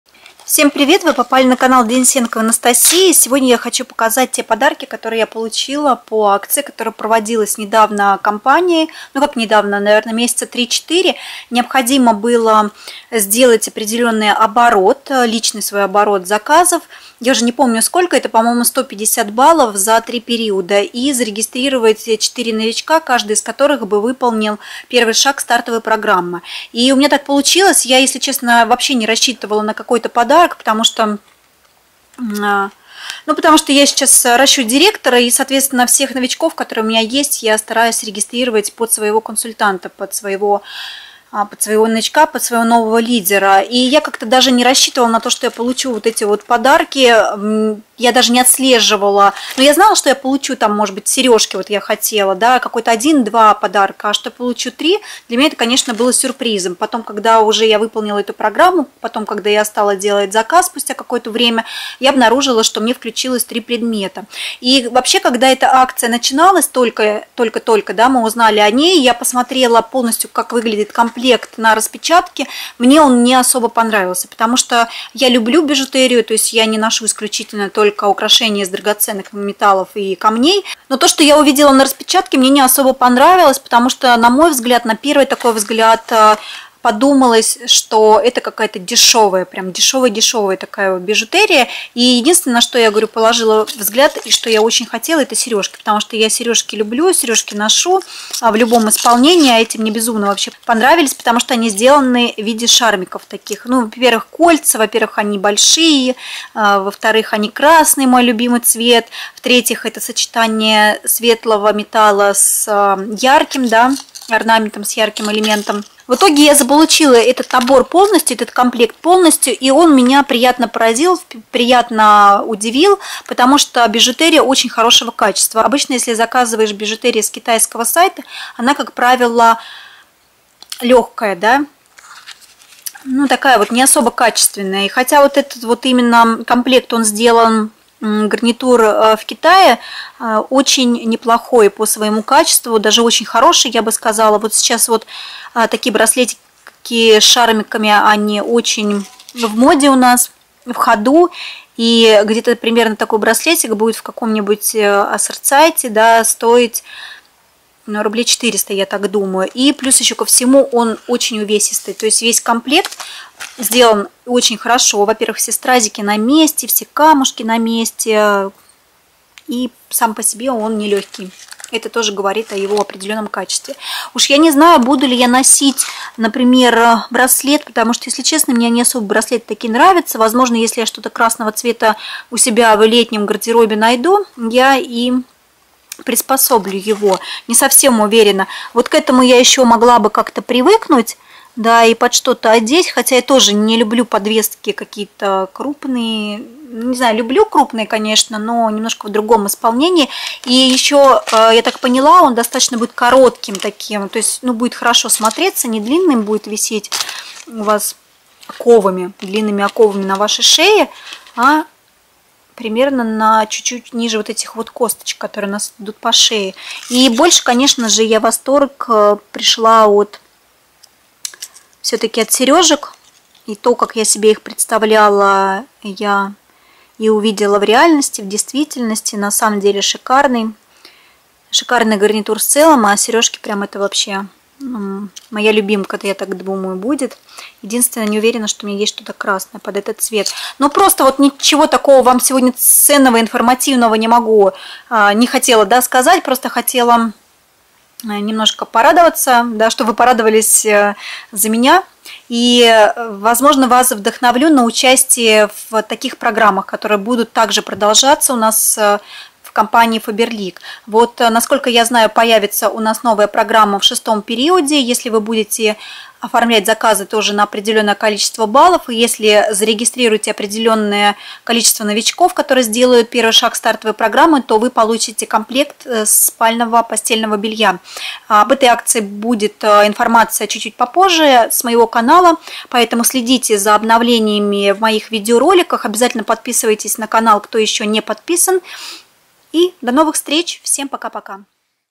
МОЯ ПРЕЛЕСТЬ...
Этой прелести вы не увидите в каталоге, потому что гарнитур был выполнен эксклюзивно для участников программы

Представительство фаберлик в Саратове:
Тархова 31A (п.Солнечный)

фаберлик Саратов